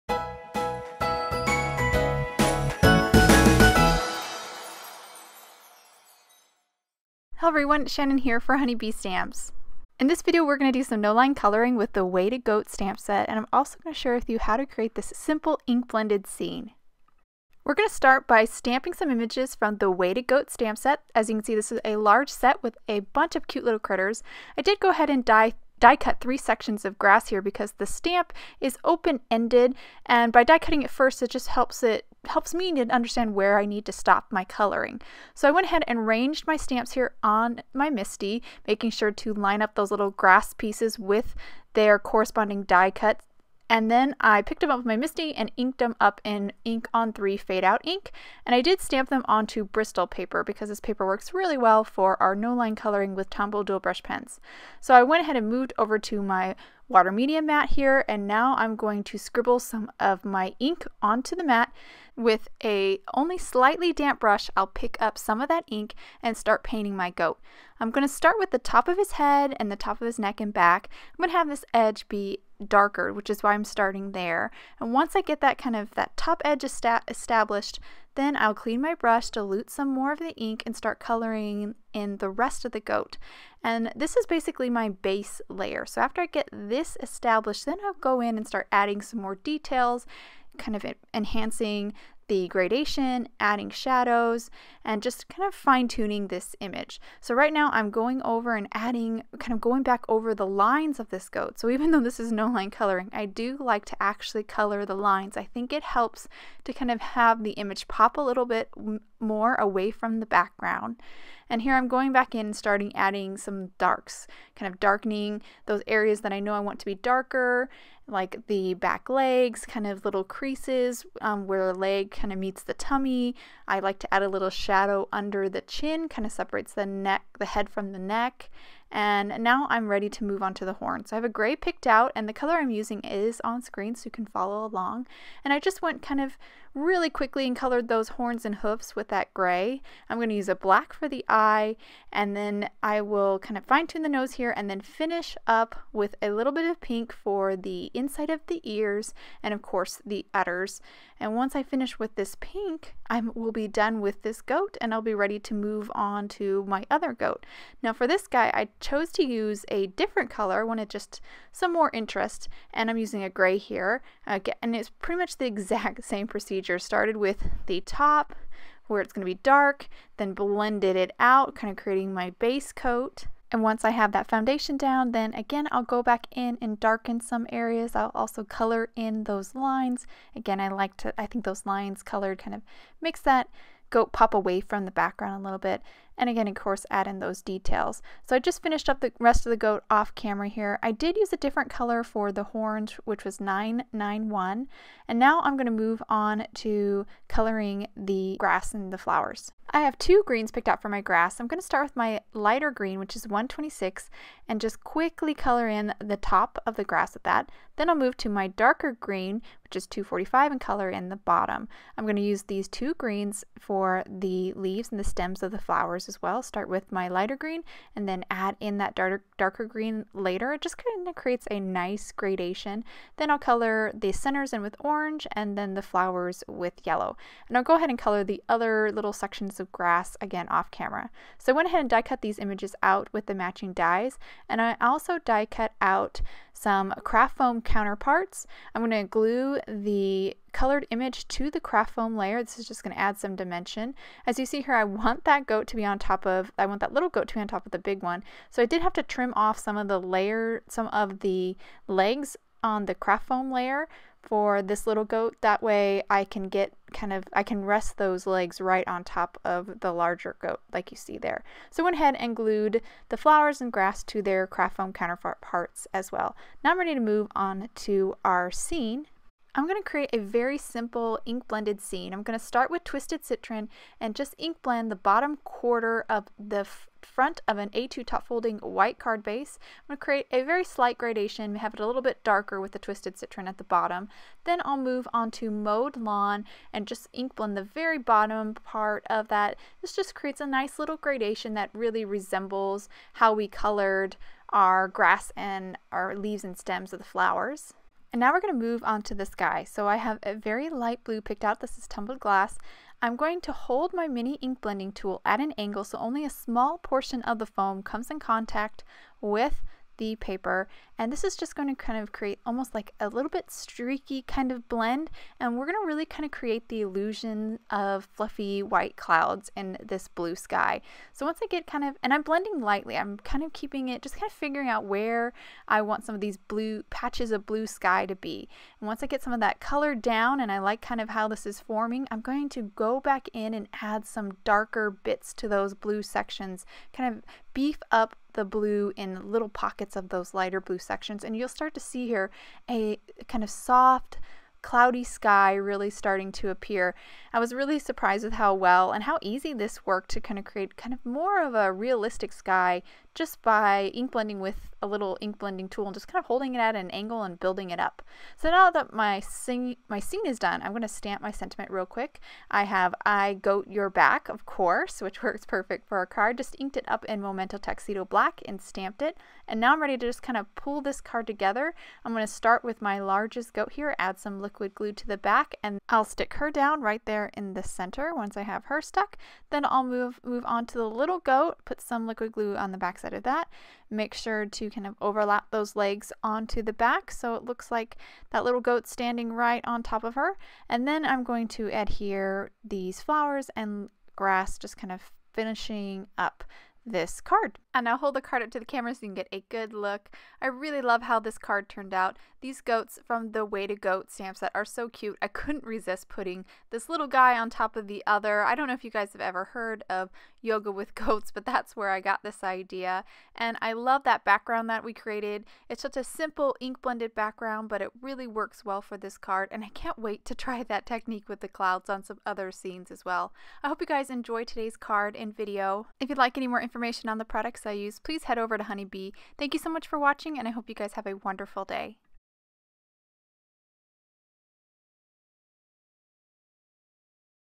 No-Line Coloring with Simple Ink Blended Scene
In this video Channin no-line watercolors some images from the Way to Goat Stamp Set to create this adorable card! Also watch to see how she creates this incredible sky simply with ink blending!

• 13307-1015 - Strathmore 300 Series Bristol Board Pads - BLICK art materials

• Tombow Dual Brush Pens and Sets | BLICK Art Materials

• White Craft Foam
[ Amazon US ]